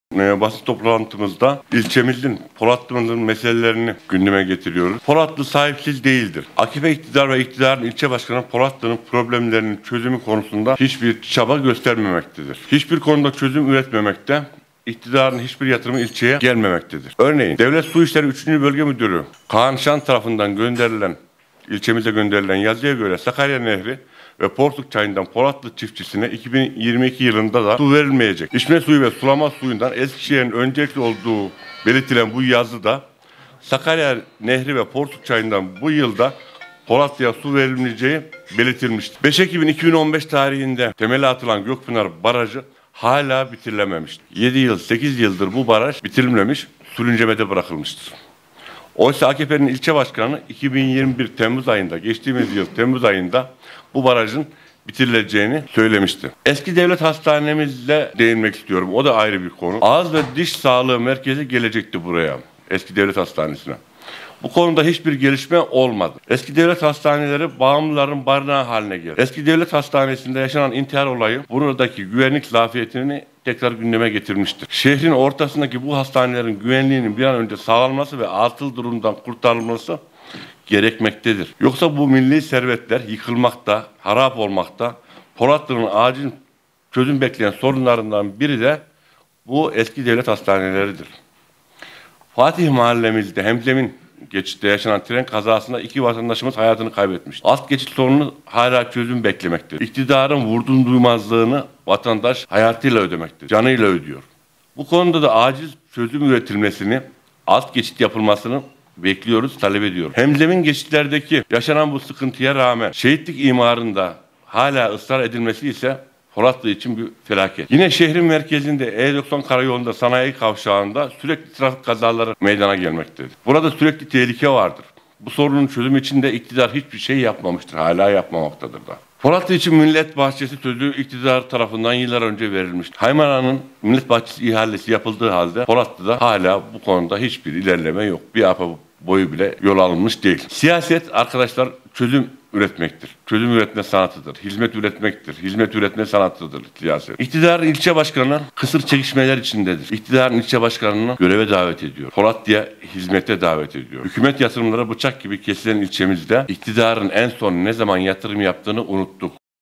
DURUKAN: “İKTİDARIN HİÇBİR YATIRIMI İLÇEYE GELMEMEKTEDİR”
İYİ Parti Polatlı İlçe Başkanı Muharrem Durukan, Polatlı gündemine dair bir basın toplantısı düzenledi. Polatlı’nın sahipsiz olmadığını dile getiren Durukan, Polatlı’nın bir türlü çözülemeyen sorunlarını dile getirdi. AK Parti iktidarı ve İktidarın ilçe Başkanının Polatlı’nın problemlerinin çözümü konusunda hiçbir çaba göstermediğini söyleyen Durukan, iktidar tarafından Polatlı’ya hiçbir yatırımın gelmediğini ifade etti. Durukan ayrıca Polatlı Devlet Hastanesindeki doktor açığına da tepki gösterdi.